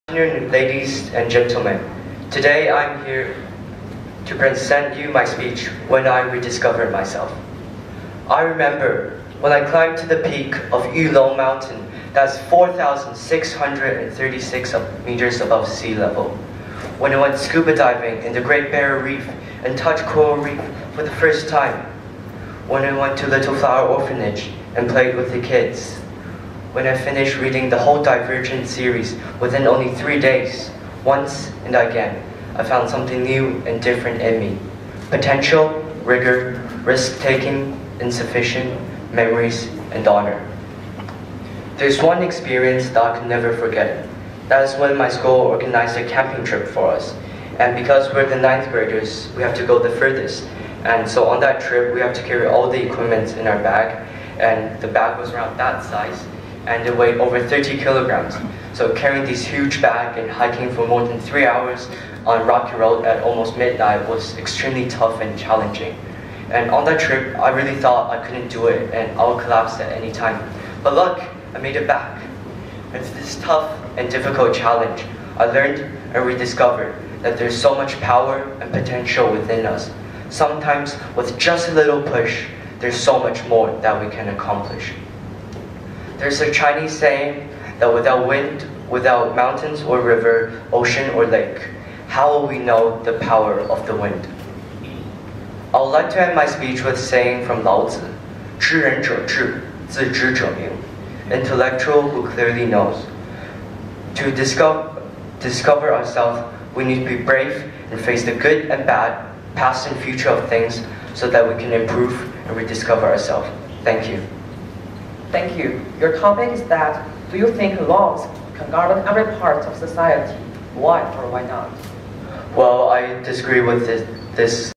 13th China Daily “21st Century Cup” National High School and Primary School English Speaking Competi
I won the runner-up prize!